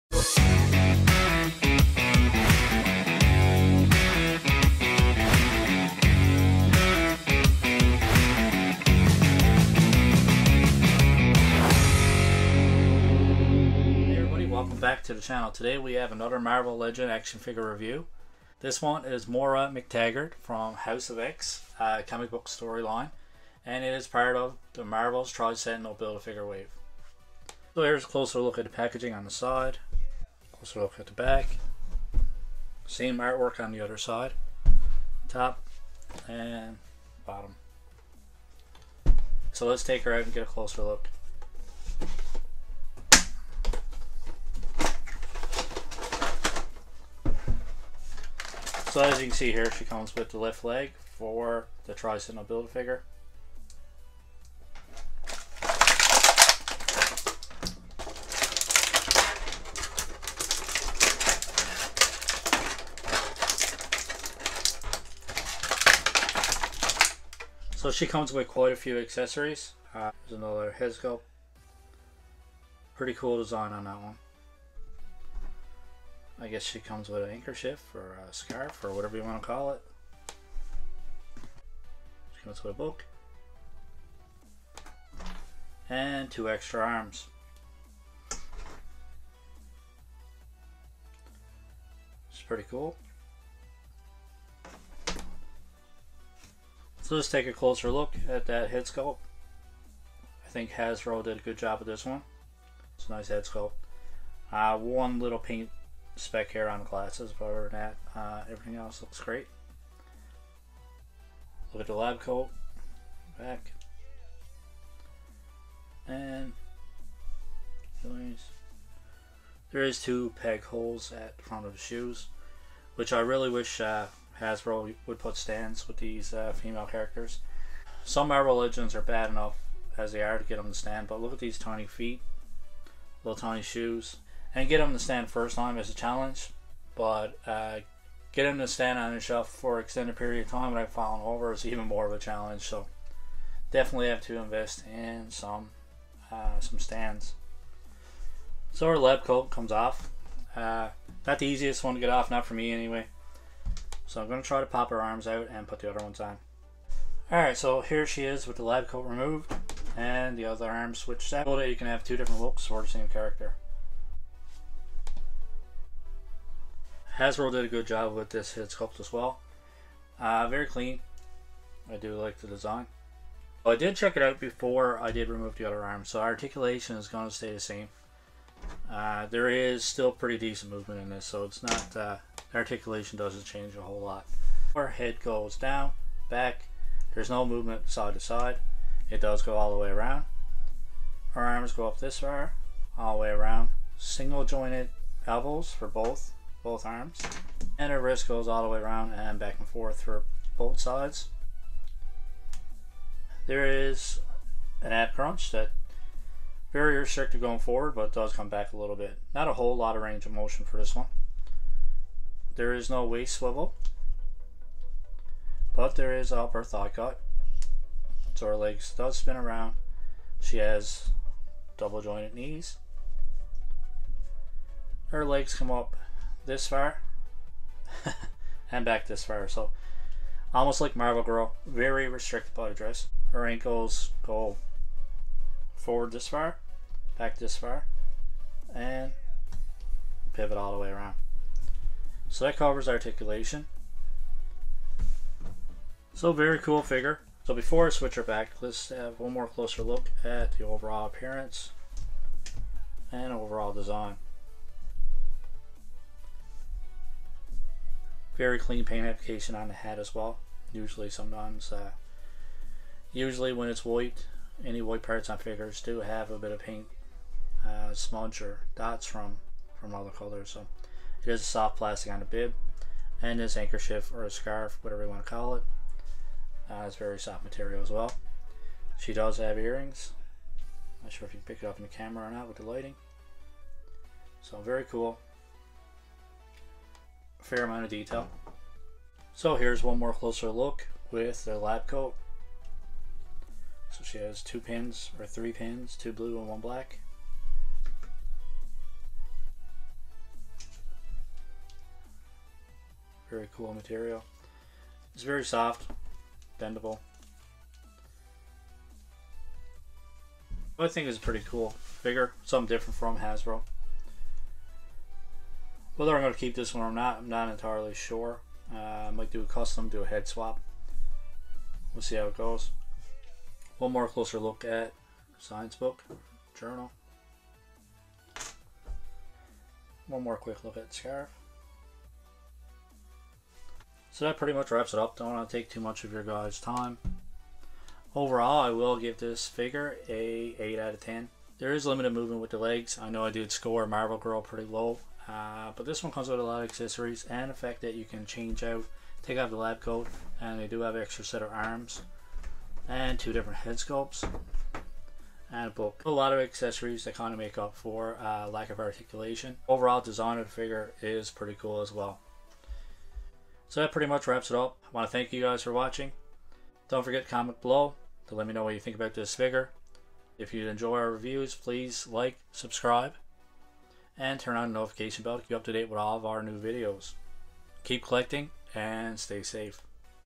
Marvel Legends Moira Mactaggert House of X Tri-Sentinel BAF unboxing and review
Hey everyone, check out our unboxing and review of the 6” Hasbro marvel legends Moira Mactaggert. This figure is from the House of X wave which builds the Tri-Sentinel Build a Figure. Visually it is a pretty nice looking figure, like Marvel Girl if you are big into posing and photography then this may not be a figure for you.